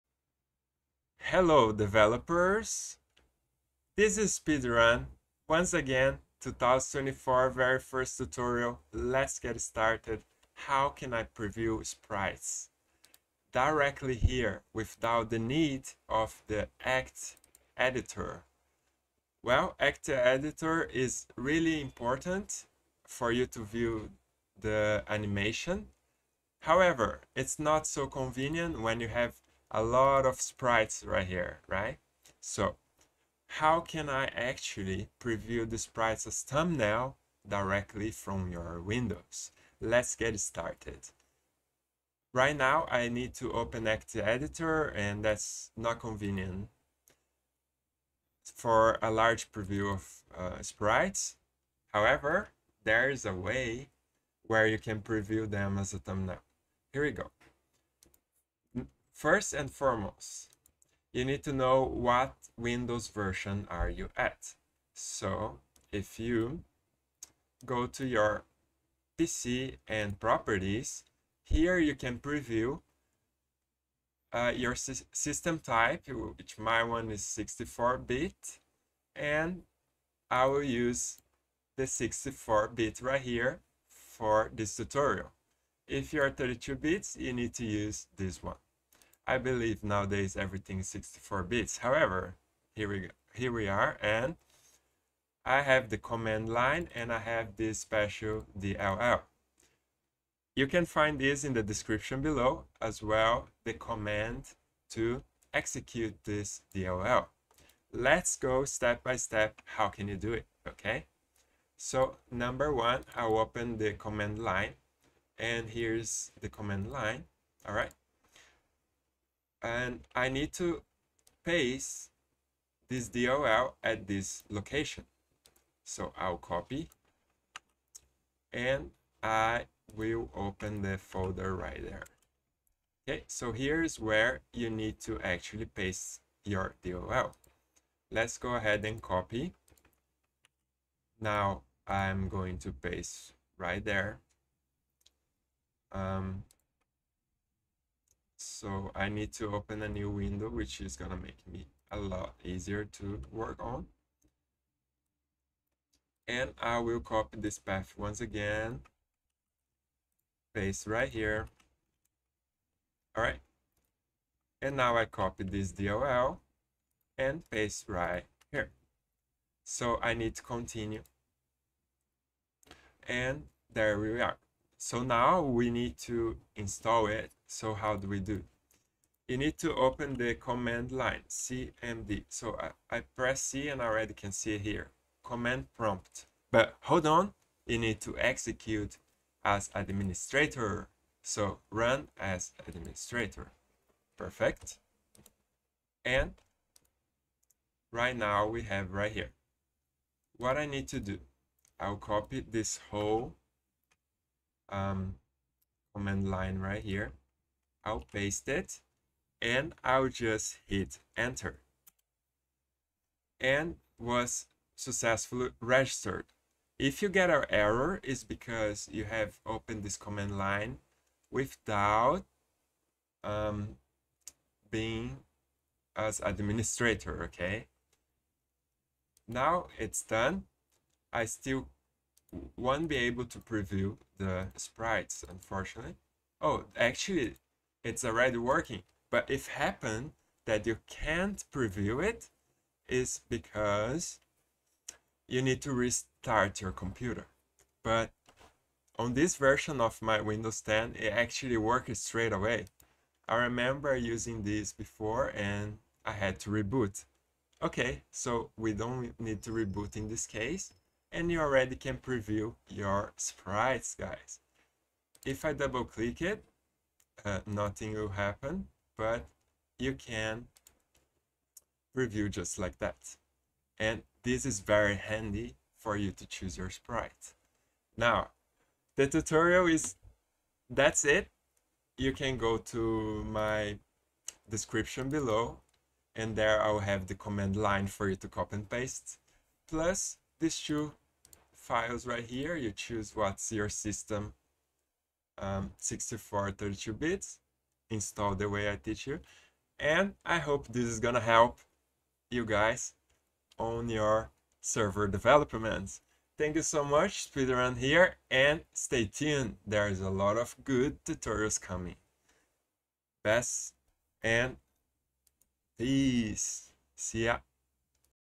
Tutorial - Preview Ragnarok Online Sprite as Thumbnail
👉Try respectRO

CMD Line:
regsvr32 "C:\Windows\System32\WeeThumbnail.dll"

Ragnarok Online WeeThumbnail Sprite Preview .spr with .dll WeeThumbnail.dll - Tutorial by Speedrun